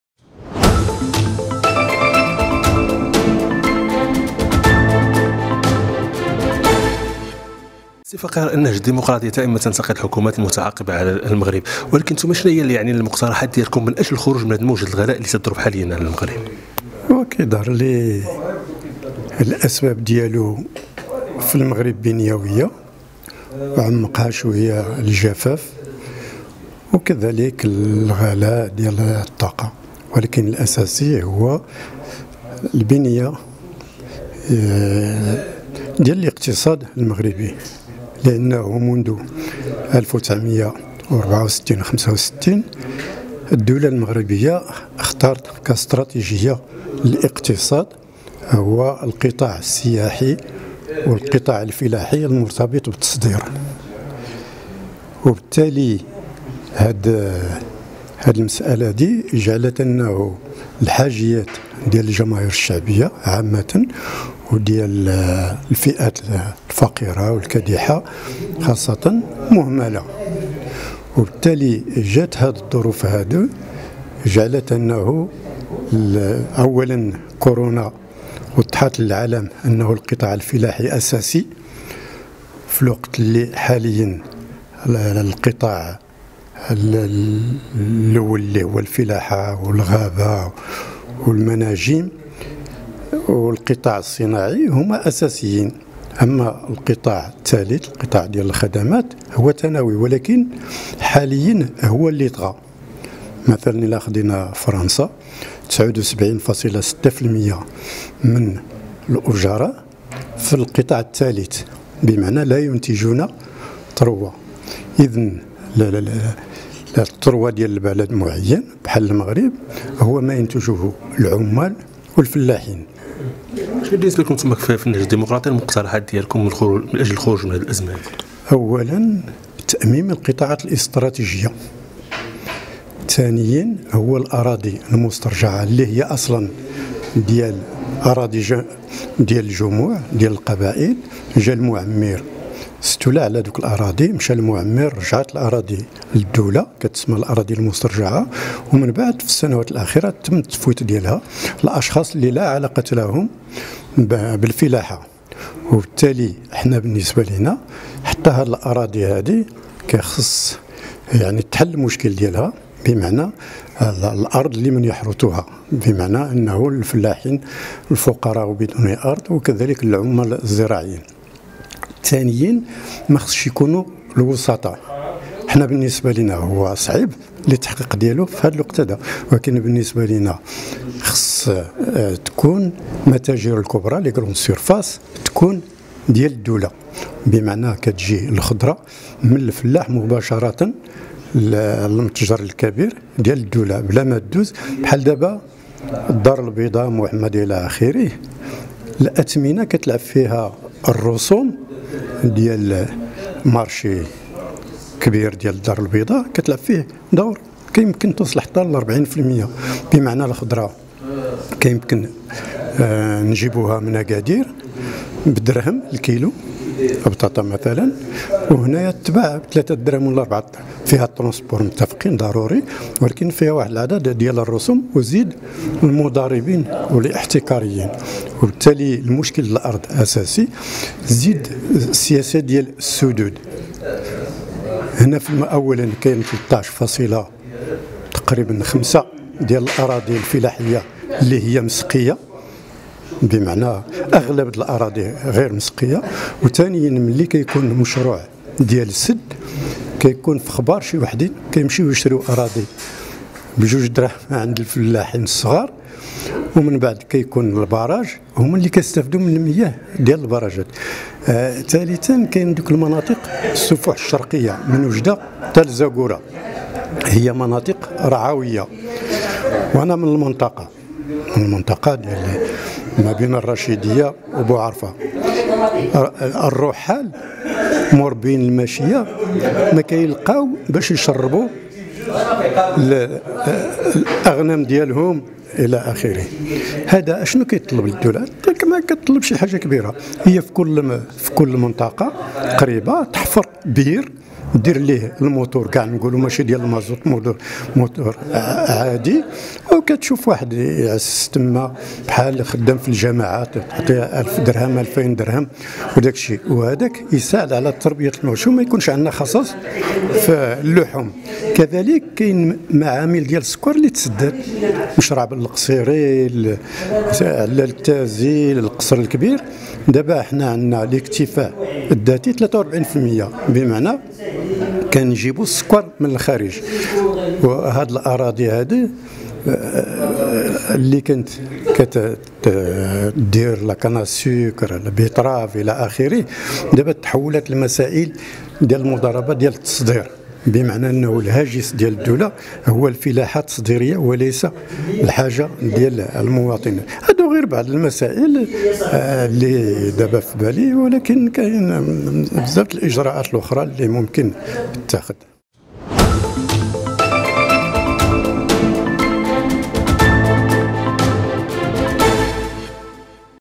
هذه وصفة النهج الديمقراطي لتجاوز أزمة الغلاء
في هذا الفيديو ستتعرف على وصفة النهج الديمقراطي من أجل تجاوز أزمة الغلاء التي يعرفها المغرب منذ شهور وأهم الإجراءات التي يجب اتخاذها للحد من هذه الموجة